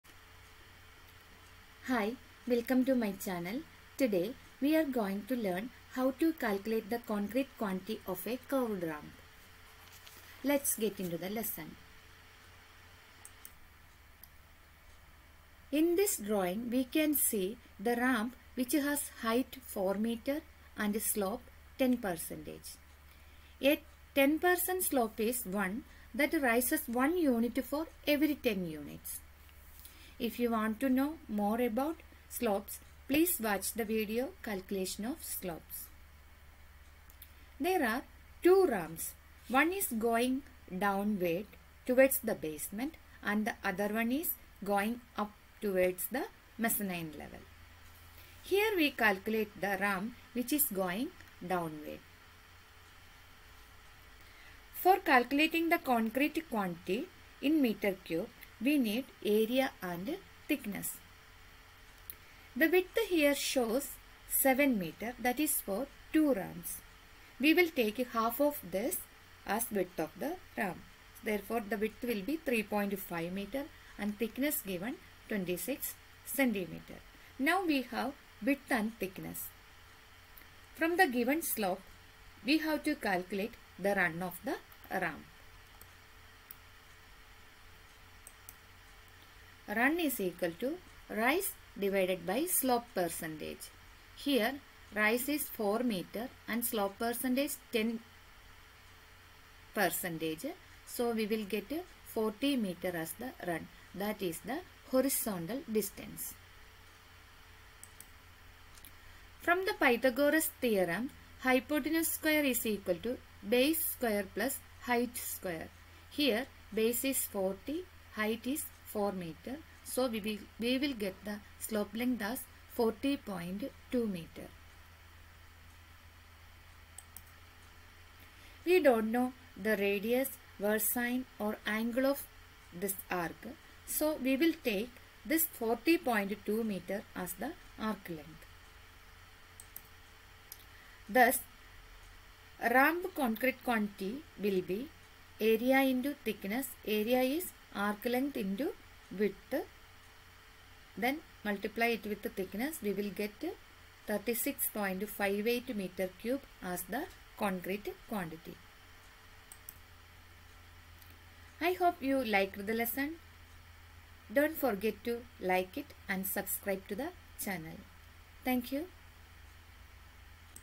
CONCRETE QUANTITY OF CURVED RAMP
In this lesson we will learn how to calculate concrete quantity of curved ramp